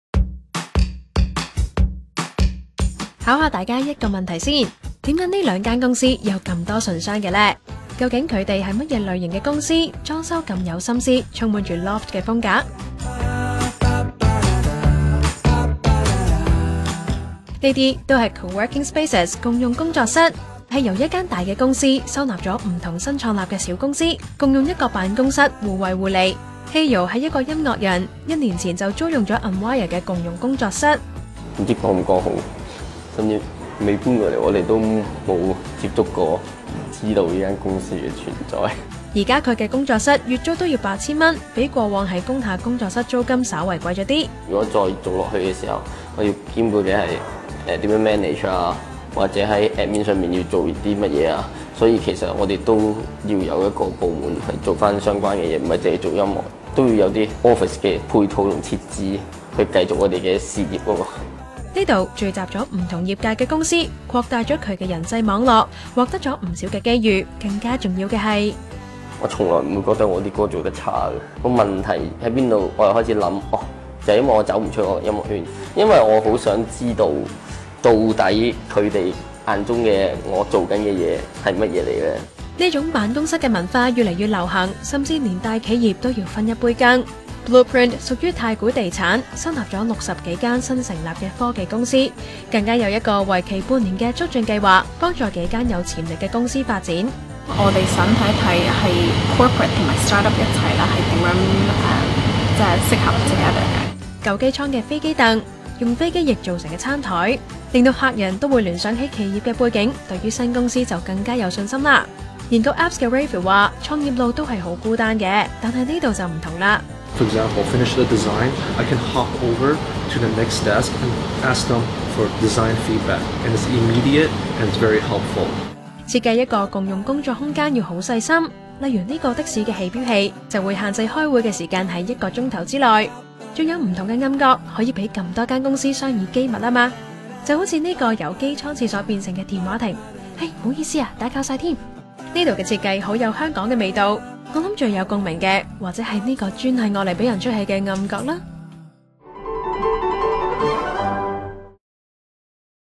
共用Office創業不孤單 分租互助蚊型公司最啱
【專題籽：胚芽故事】
共用工作空間（Coworking Space），是個讓不同團隊一起工作的社群平台，尤其是新成立的小公司。記者自認「大鄉里」，事前對這種工作形態聞所未聞，來到創業加速計劃blueprint的豪華辦公室，“welcoming community manager”Debbie邀我一同踏上接待處前的腳掌圖案，牆上旋即亮起"Coworking"的字樣。原來，這裏不止「一間」公司，而是由眾多本地蚊型科技公司所組成的「巨蚊」，剛才兩個腳踏，正是比喻大公司與細公司的互助精神。